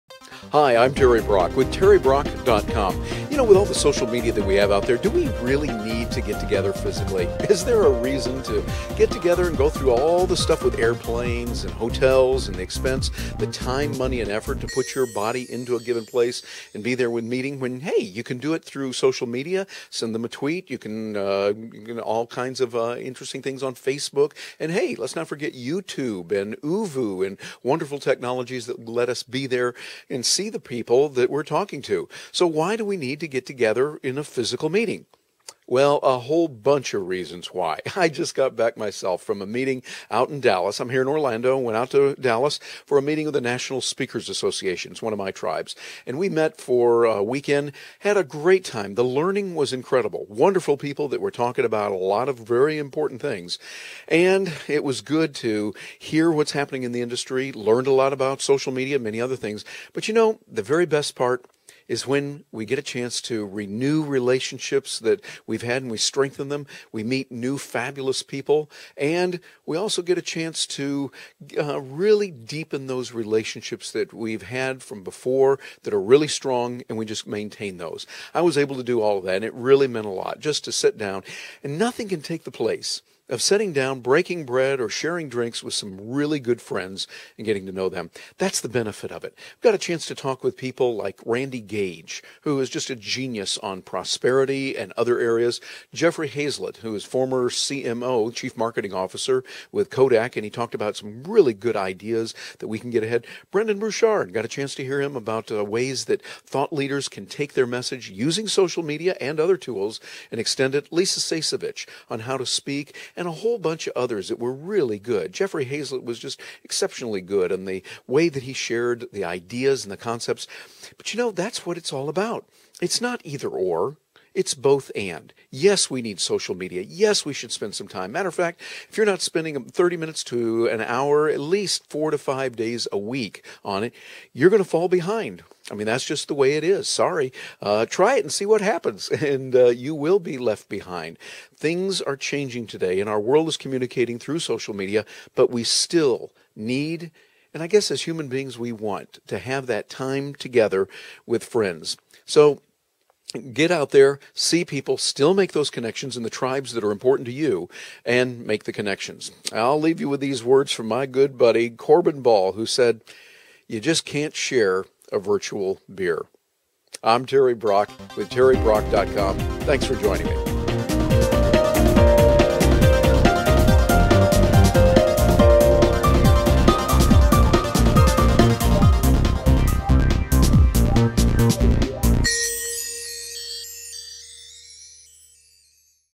Are Meetings Really Necessary w/Social Media?
Do we still need to have physical meetings now that Social Media is here?  Why travel when you can use Skype, Oovoo, Facebook, Twitter, etc. to connect?  Watch this video and discover.